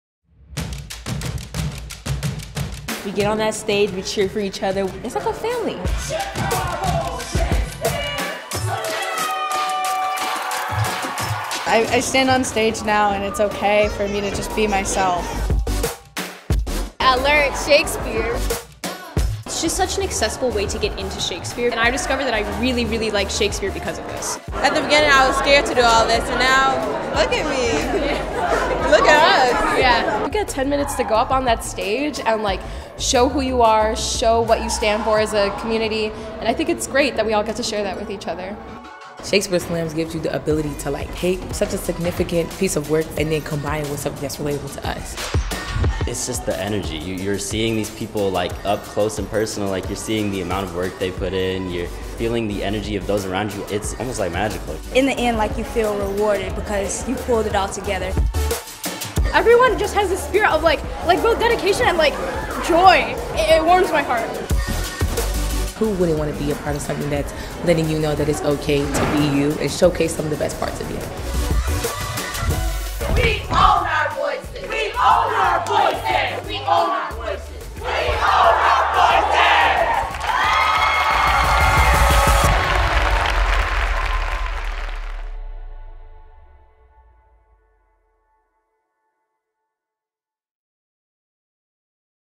SLAM 22/23 | “I stand onstage and it’s OK to just be myself”
Students pushing beyond their comfort zones to bring Shakespeare’s stories to new life onstage. A DJ dropping beats to dance moves inspired by Richard III. And a community enthusiastically cheering one another in a resounding wave of support. This is Chicago Shakespeare Slam.

Teams representing thirty high schools from across the region came together to celebrate the power of language—and their own voices—in the sixth annual SLAM. A series of Saturday workshops and regional preliminary bouts throughout the fall culminated in a spirited, fun-filled Final Bout in the Courtyard Theater. SLAM encourages students to strive for the goals of “Be Seen, Be Heard, Be Creative, Take Risks, and Play as an Ensemble” as they tell their own stories in conversation with Shakespeare’s words.